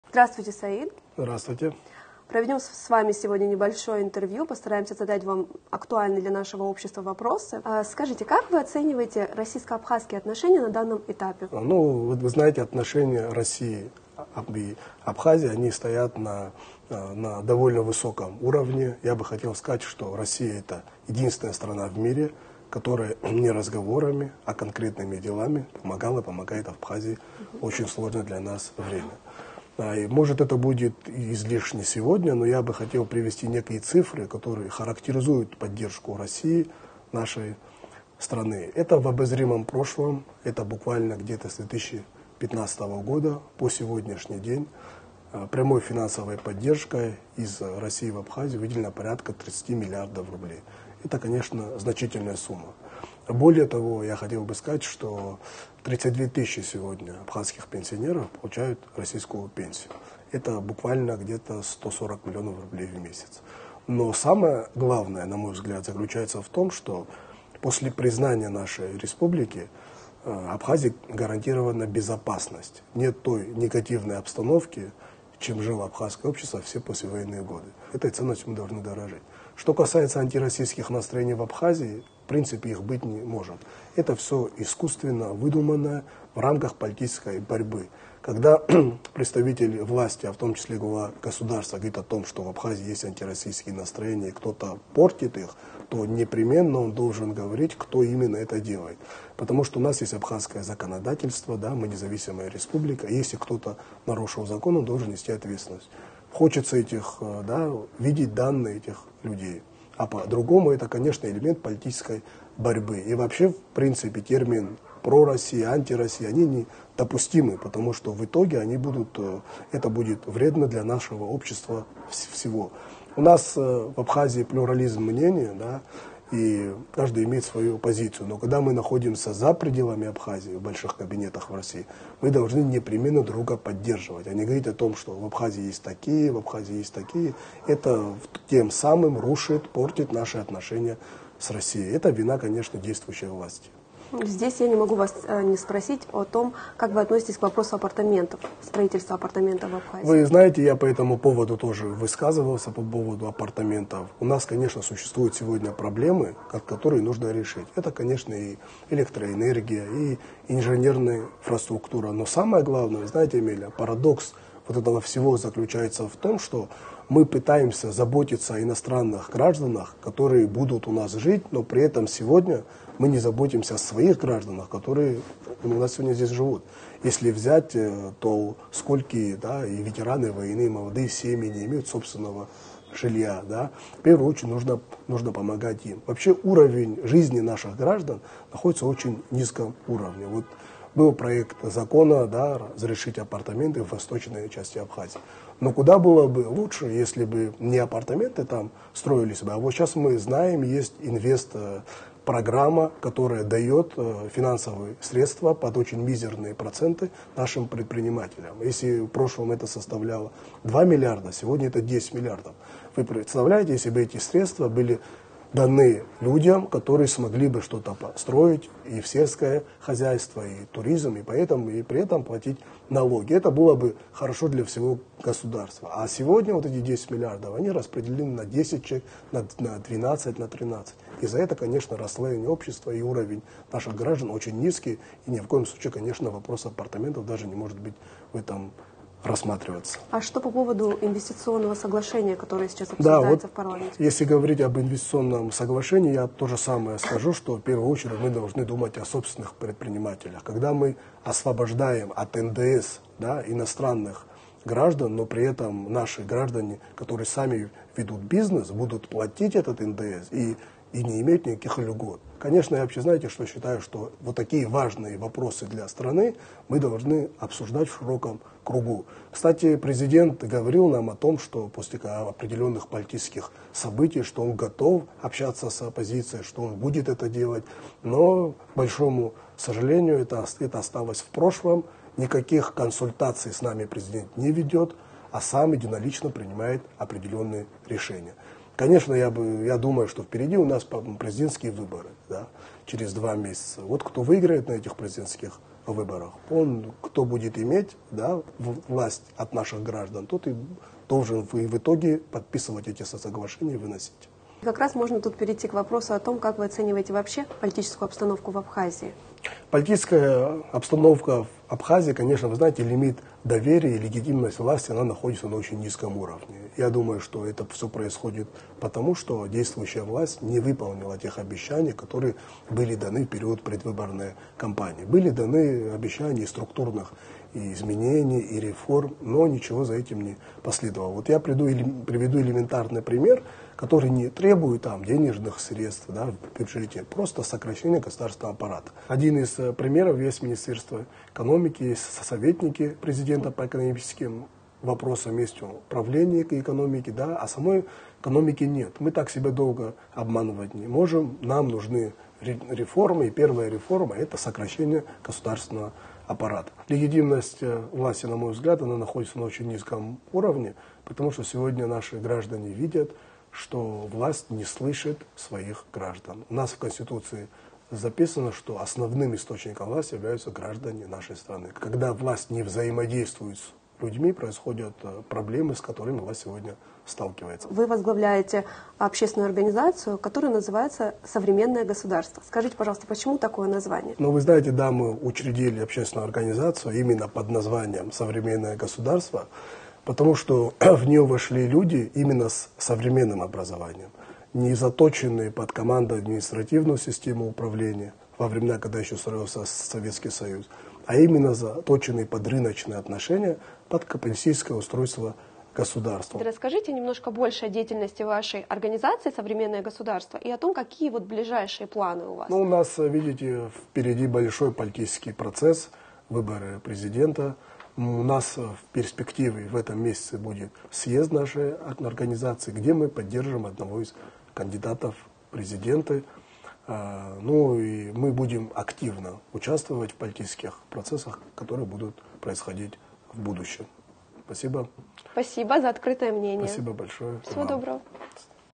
Саид Бутба: Парадокс в том, что мы пытаемся заботиться об русских гражданах, которые будут у на
Саид Бутба: Парадокс в том, что мы пытаемся заботиться об русских гражданах, которые будут у нас жить, но при этом сегодня мы не заботимся о своих абхазах